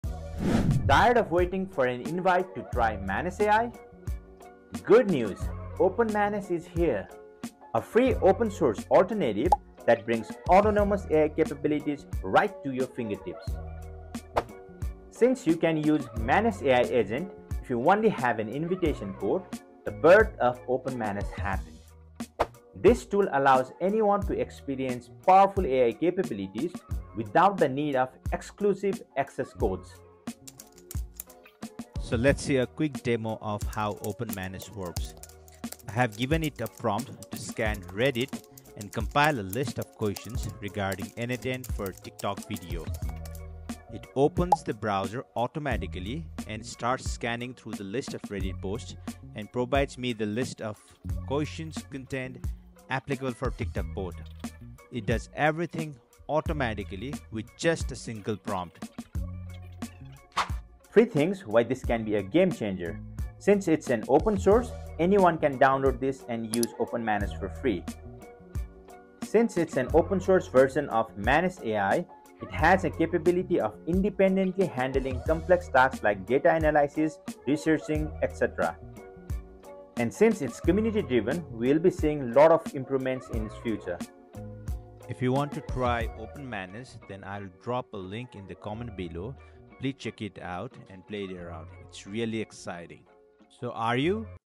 OpenManus: Fully free open source. An alternative to Manus, a general AI Agent.
Manus is incredible, but OpenManus can achieve any idea without an Invite Code 🛫!

Enjoy your own agent with OpenManus!